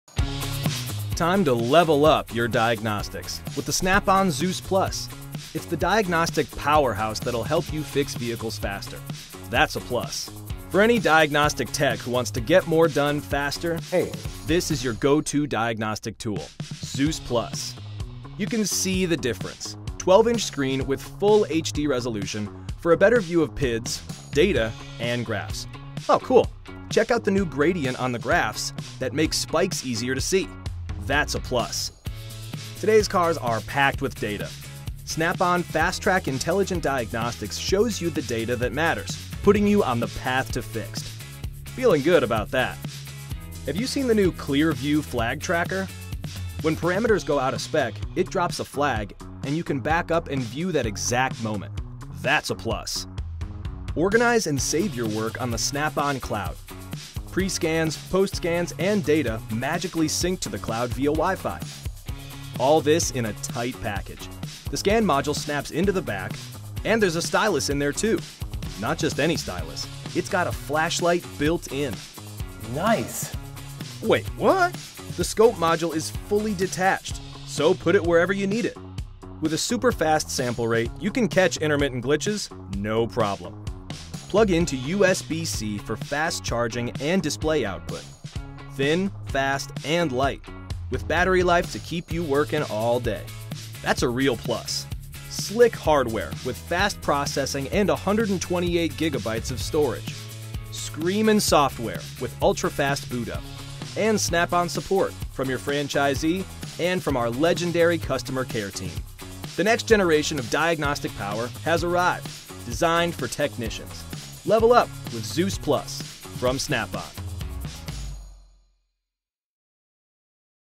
THE NEW ZEUS+™ | Snap-on Diagnostics UK
THE SUPER-POWERFUL, SUPER-INTELLIGENT, SUPER-AT-EVERYTHING DIAGNOSTIC TOOL.
Driven by Fast-Track Intelligent Diagnostics, the ZEUS+ provides you with the information you need to diagnose, repair and manage any issue – simplifying your workflow and increasing your efficiency on every job.

New Here?